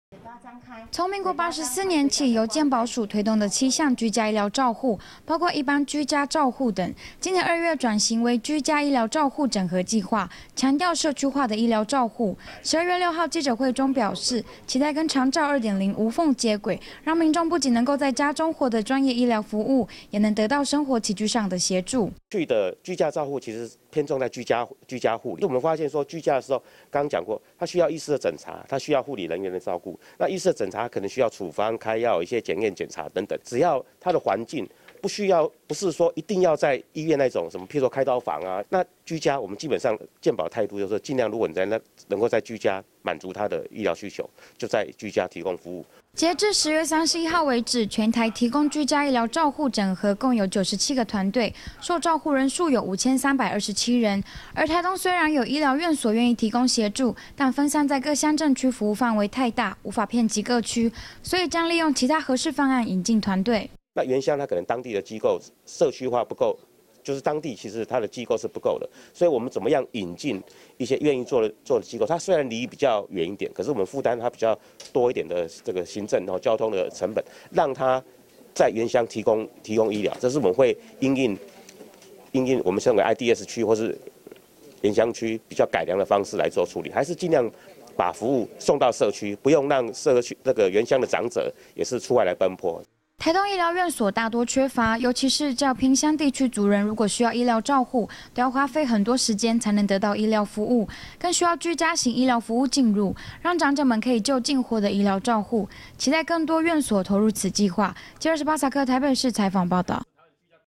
健保署推居家醫療照護 台東偏鄉仍缺乏2016-12-06 TITV 原視新聞
從民國84年起由健保署推動的七項居家醫療照護，包括一般居家照護等，今年2月轉型為「居家醫療照護整合計畫」，強調社區化的醫療照護，１２月６號記者會中表示，期待跟長照2.0無縫接軌，讓民眾不僅能夠在家中獲得專業醫療服務，也能得到生活起居上的協助。

截至10月31號為止，全台提供居家醫療整合照護共有97個團隊，照護人數有５３２７人，而台東雖然有醫療院所願意提供協助，但分散在各鄉鎮區服務範圍太大，無法遍及各區，所以將利用其他合適方案引進團隊。

台東醫療院所大多缺乏，尤其是較偏鄉地區族人如果需要醫療照護，都要花費很多時間才能得到醫療服務，更需要居家型醫療服務進入，讓長者們可以就近獲得醫療照護，期待更多院所投入此計畫。